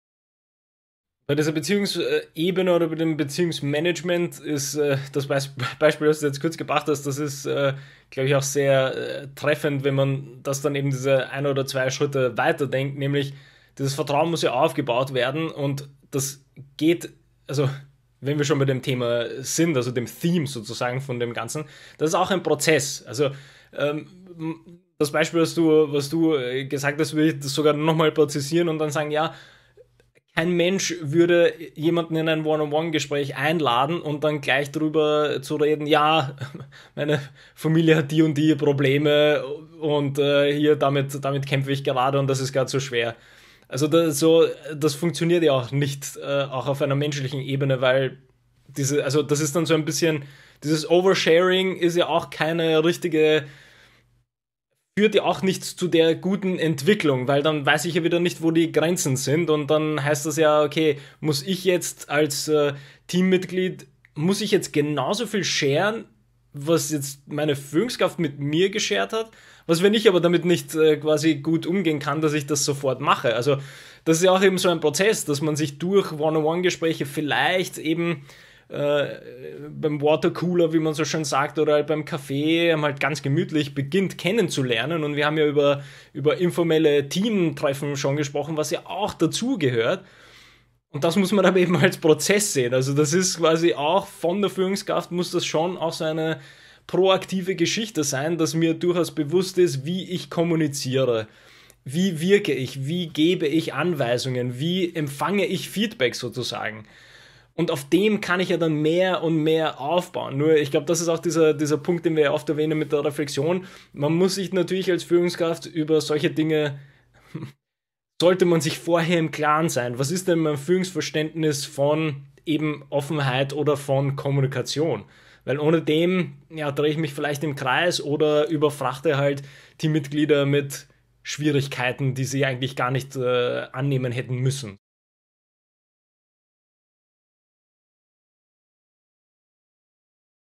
Beziehungsmanagement & Vertrauen als Prozess
In diesem kurzen Clip spricht Bence über den gemeinsamen Prozess des Kennenlernens zwischen Führungskraft und dem Team.